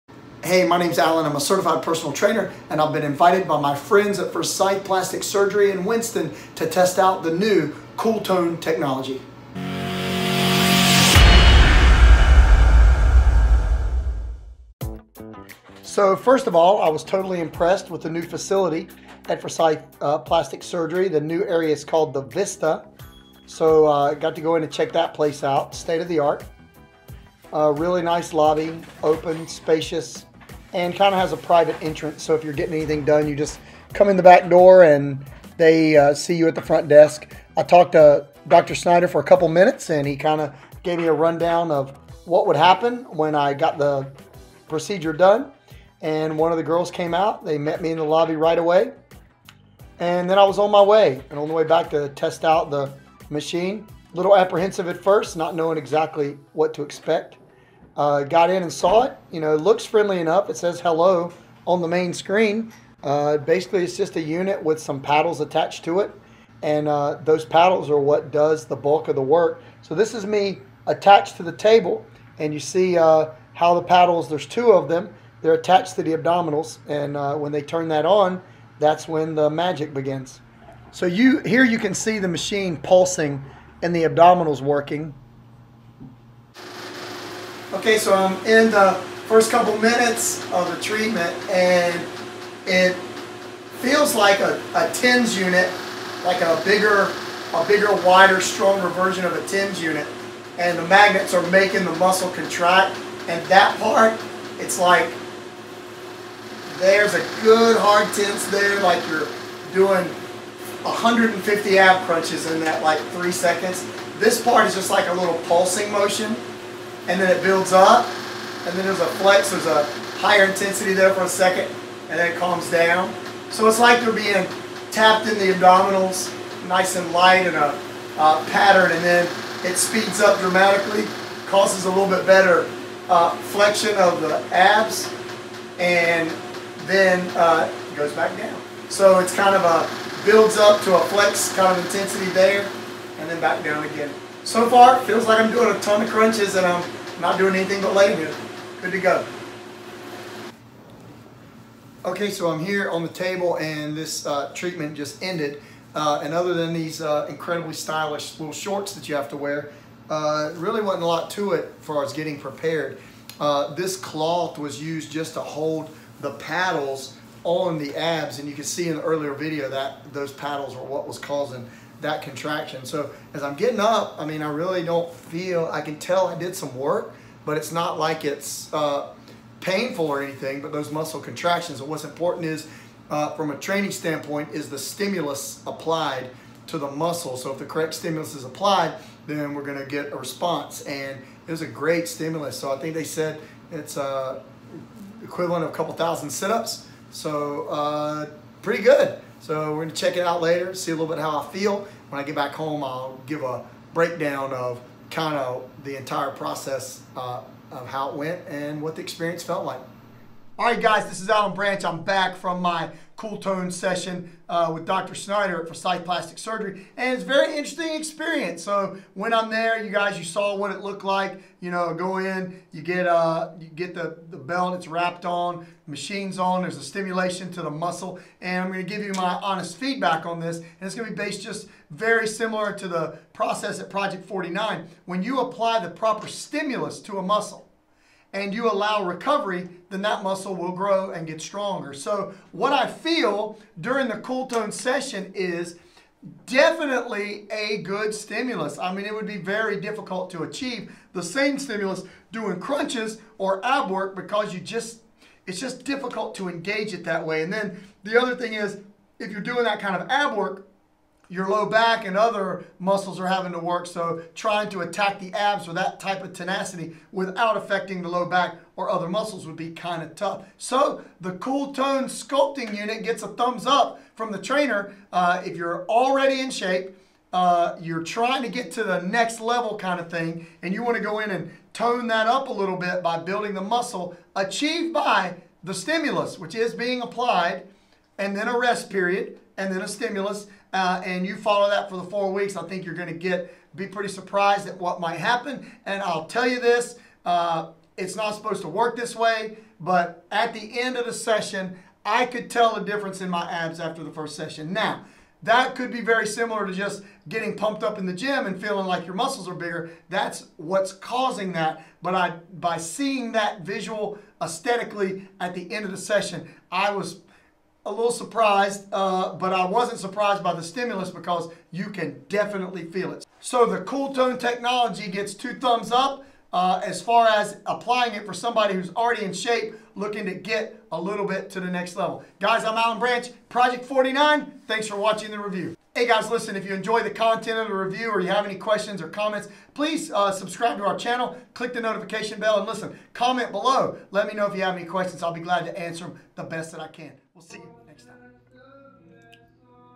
Cooltone by Coolsculpting REVIEW by a personal trainer
Gimmick or nice addition to a toning routine for abs, glutes or thighs? My pal Andy Schneider of Forsyth Plastic Surgery in Winston Salem NC brought me in to the put the new COOLTONE technology to the test.

Cooltone promises a toned and tighter midsection if used on the abs, so I skeptically decided to give it a go and see if any of the hype is real.